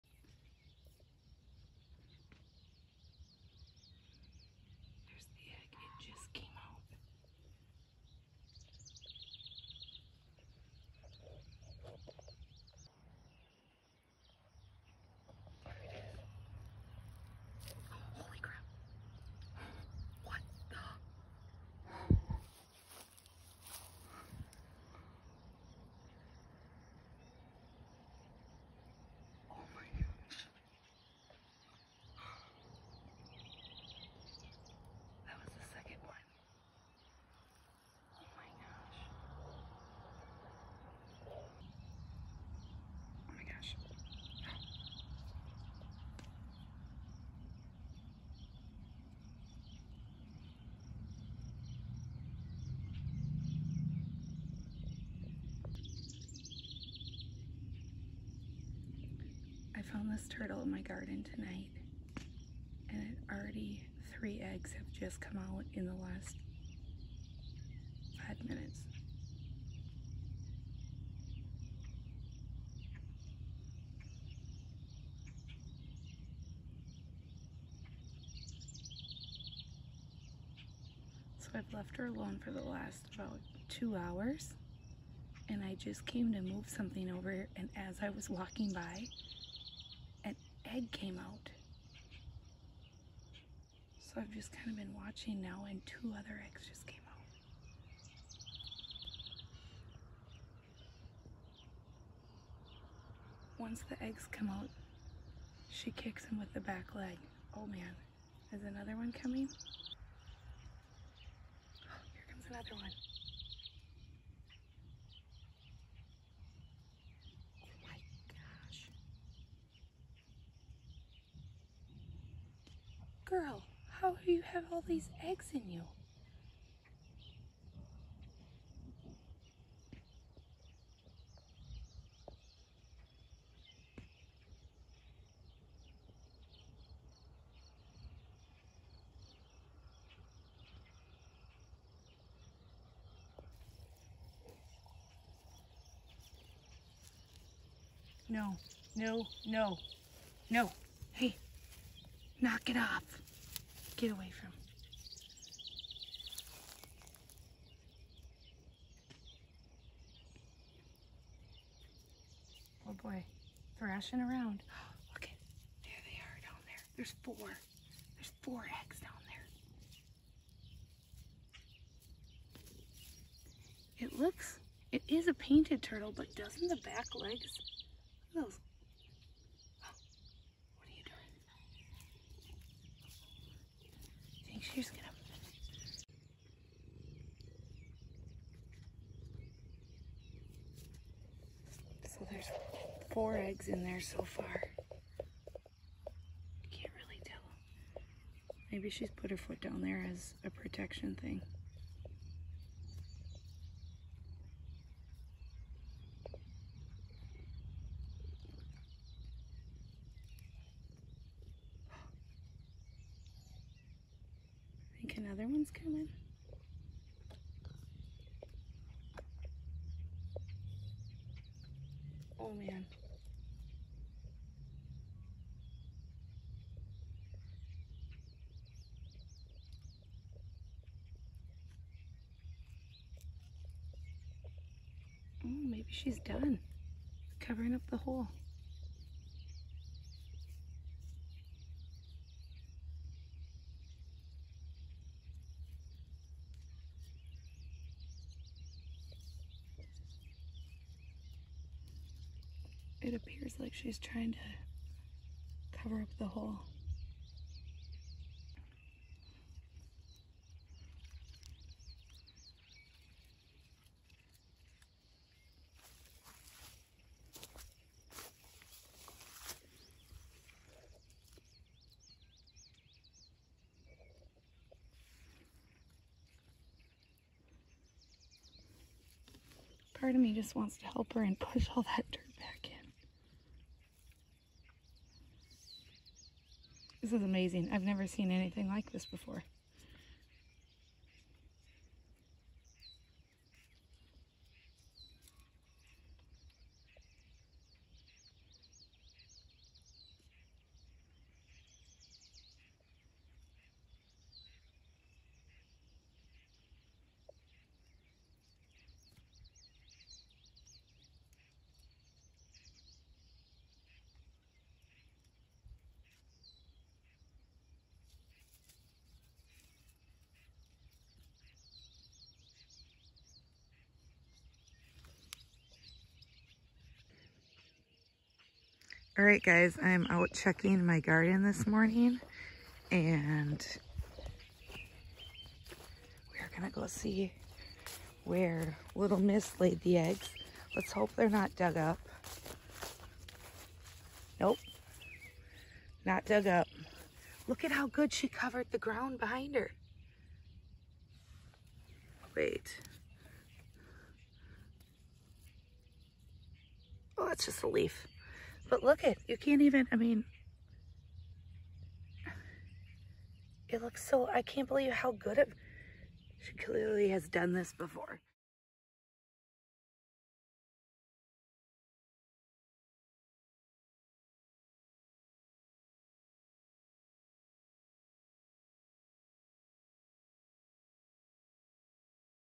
🐢 Turtle giving birth! 🥚 Painted Turtle dropping eggs.
Auntie found a turtle while she was working in her garden! Watch it drop eggs into the hole and fill the hole back with dirt. See what it looks like the next morning.